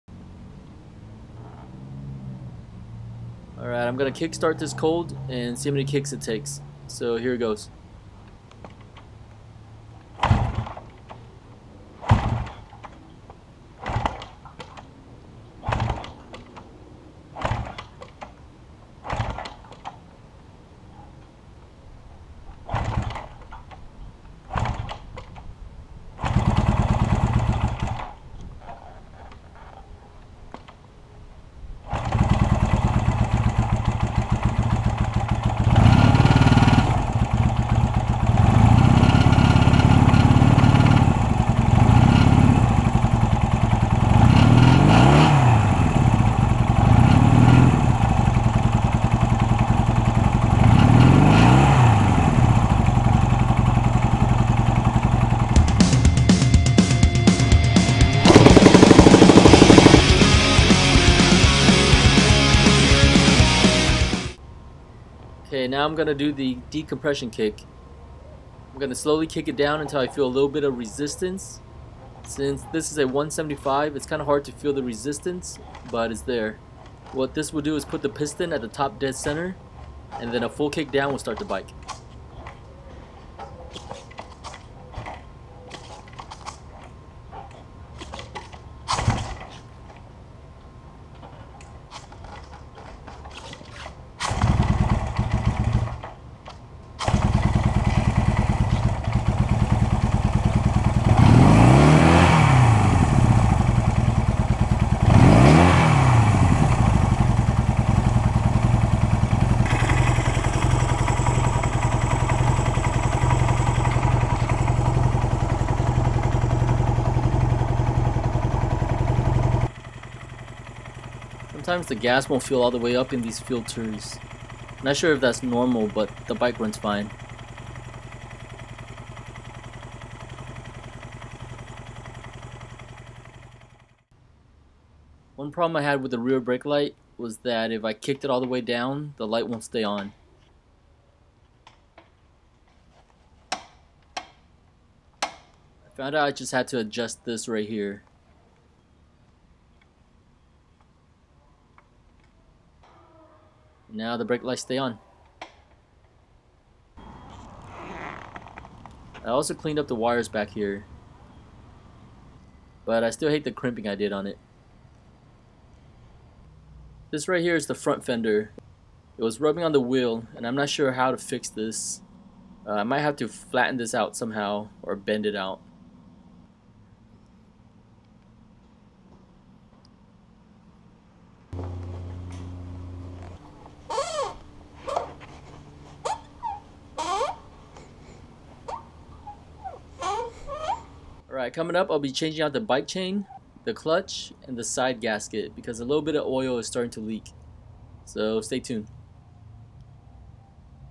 Honda CL175 Cafe Racer Pt. 15 - Decompression Kick and LED Tail Light Fix
Regular kick start Vs. Decompression kick start. The decompression kick is where we try to get the piston at the Top Dead Center. This will make the bike kick start easier.

I also show some updates to my tail light and front fender.
1971 Honda CL175 Scrambler.